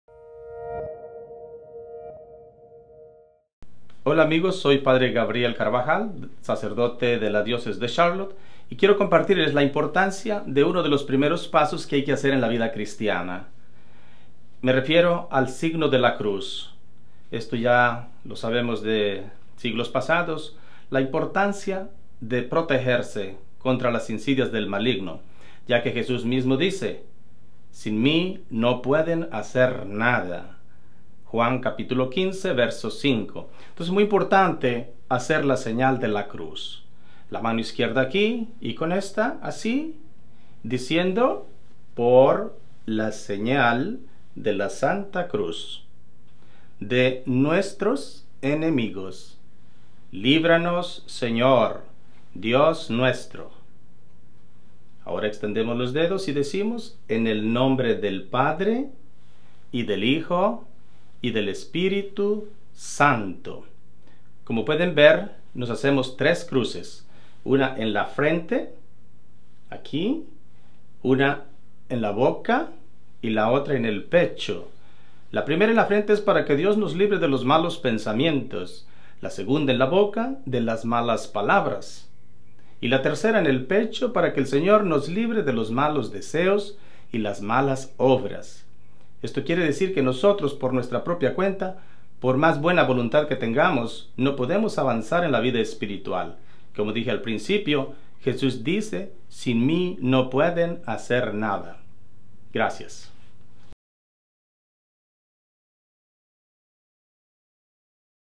¿Cómo persignarse correctamente?
Persignarse es un  signo sacramental básico para los católicos. Aprendamos cómo hacer la señal de la cruz correctamente y por qué es importante.

El Padre Gabriel Carvajal Salazar, mayor de quince hermanos, nació en México el 28 de Febrero de 1959.  Por varios años fue catequista y misionero laico en el Movimieto Apóstoles de la Palabra en México, Belice, y Guatemala.
Empezó su formación sacerdotal en el 2002, en Louisiana, New York, y finalizó los estudios teológicos en la Universidad Pontificia de México.
Fue ordenado sacerdote el 5 de junio del 2010 en Huntersville, Carolina del Norte, en donde ha sido vicario en las parroquias de St. Vincent de Paul y St. Aloysius. Actualmente sirve en la Iglesia St. Gabriel, en Charlotte, y colabora con The Catholic Company.